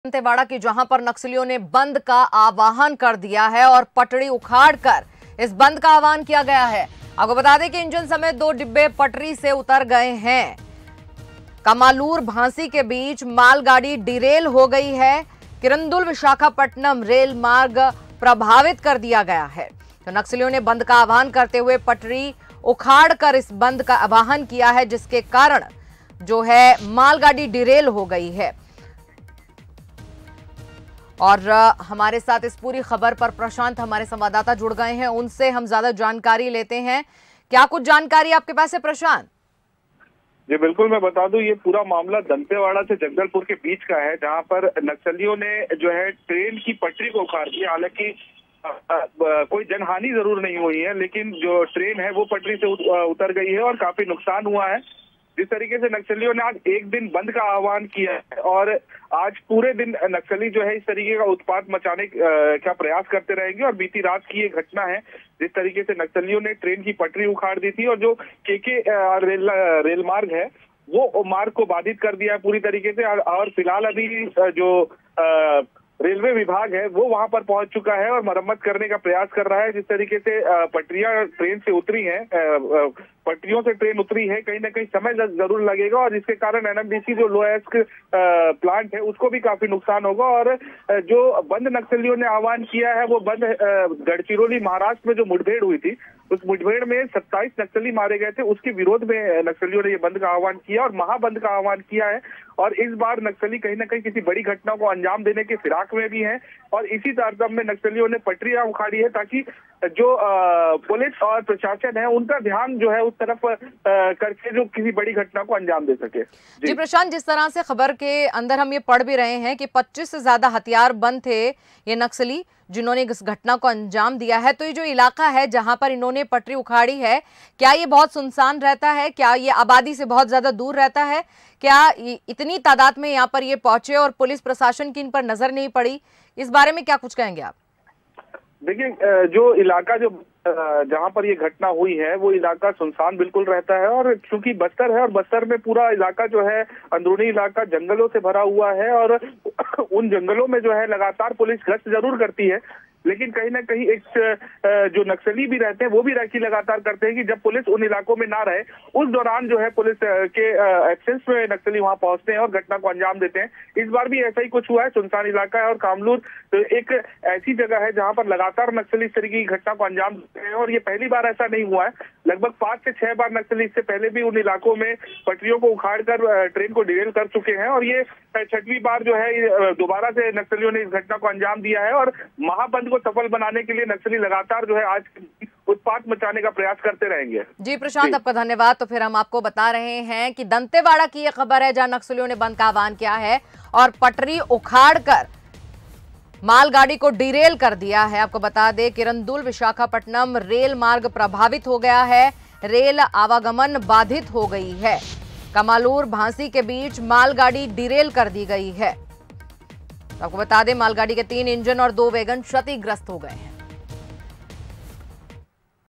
दंतेवाड़ा मे नक्सलियों का बंद आह्वान... पटरी उखाड़ी, इंजन सहित दो डिब्बे पटरी से उतरे
पटरी उखाड़कर किया बंद का आह्वान
इंजन सहित दो डिब्बे पटरी से उतरे
कमालूर-भांसी के बीच मालगाड़ी हुई डिरेल
किरंदुल-विशाखापट्टनम रेल मार्ग प्रभावित